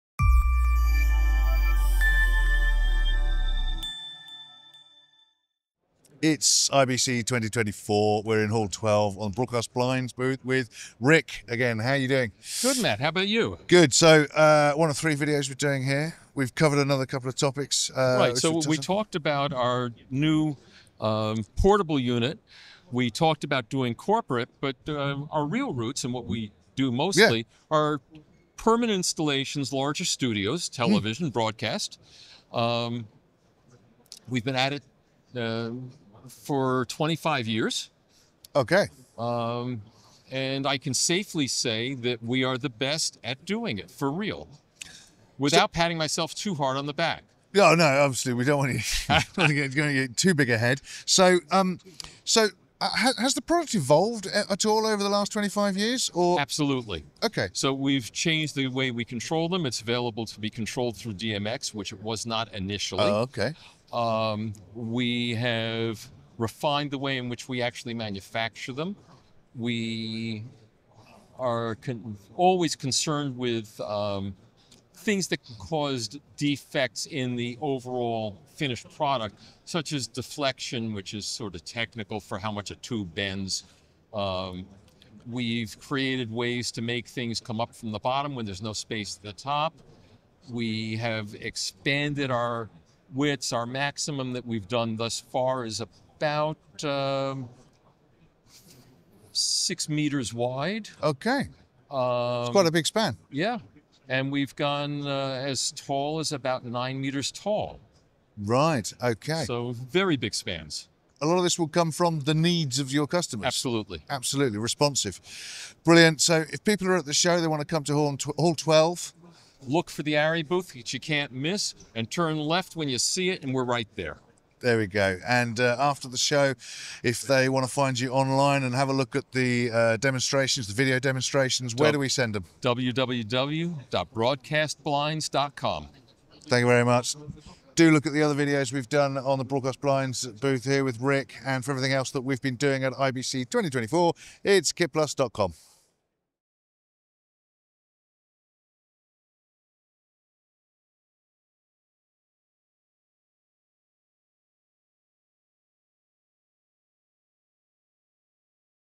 Broadcast Blinds Daylight Control for Studios at IBC 2024
At IBC 2024 we speak with Rick Smith  from Broadcast Blinds about Daylight Control for Studios